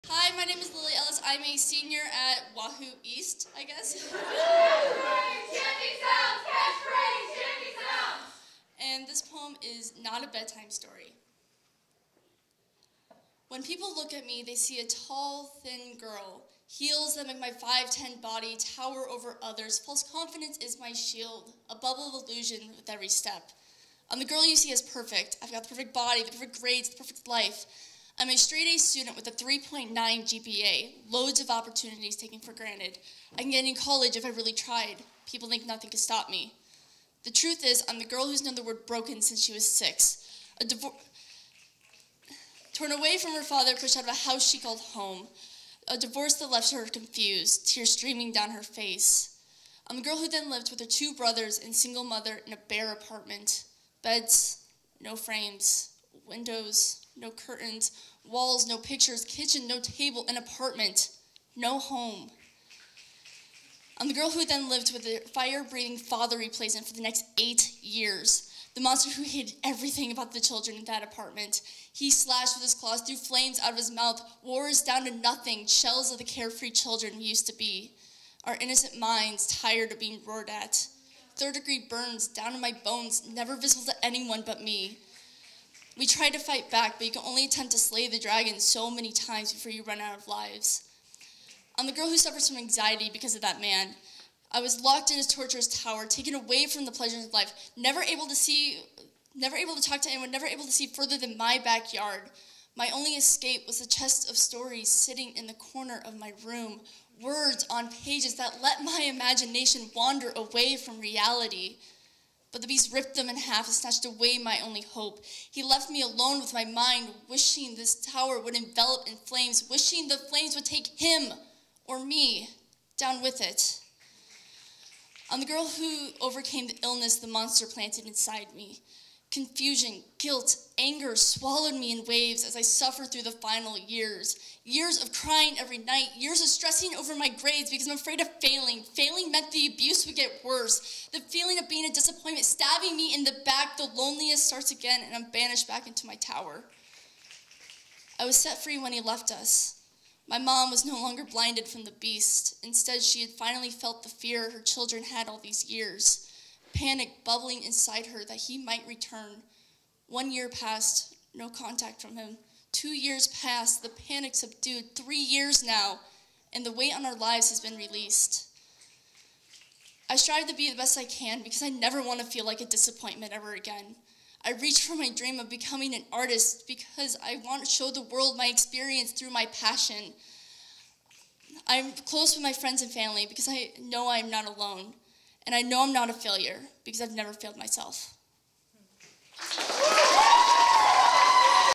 Louder Than a Bomb Great Plains, 2018: Bout 10, UNL
Louder Than a Bomb Great Plains, 2018 Bout 10 March 27 at UNL in the Nebraska Union auditorium (5:30pm)

Featuring teams from: Wahoo, Lincoln East, Lincoln Southeast,, and Lincoln Northeast